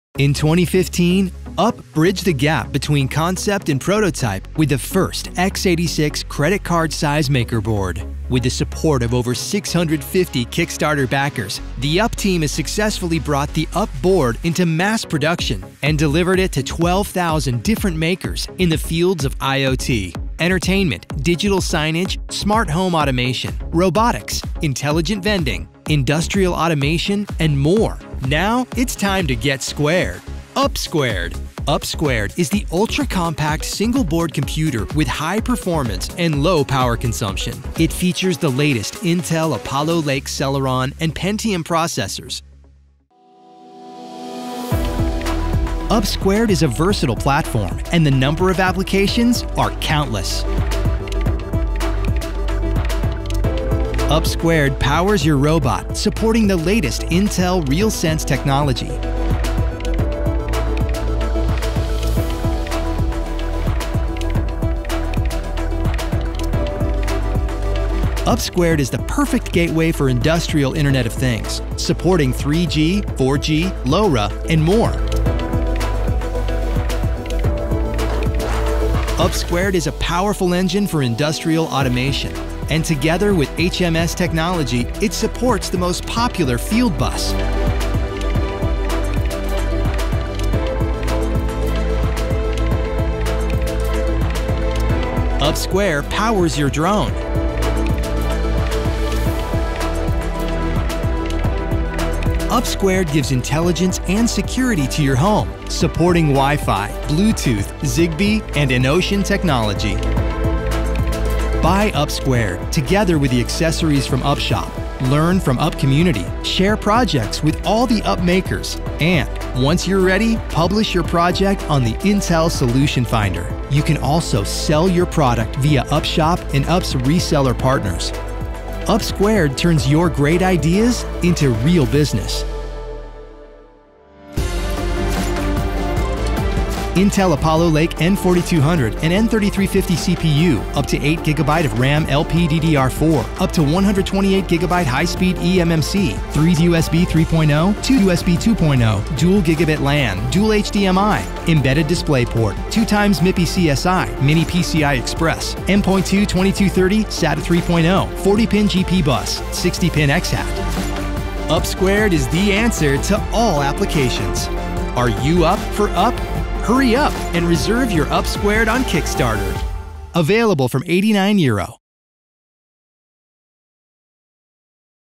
AAEON UP SQUARED - The first Pro-Maker board with Intel Apollo Lake CPU
UP Squared is the world’s fastest x86 pro-maker board

based on latest Intel platforms Apollo Lake.

UP is the bridge between your hobby

and your future business.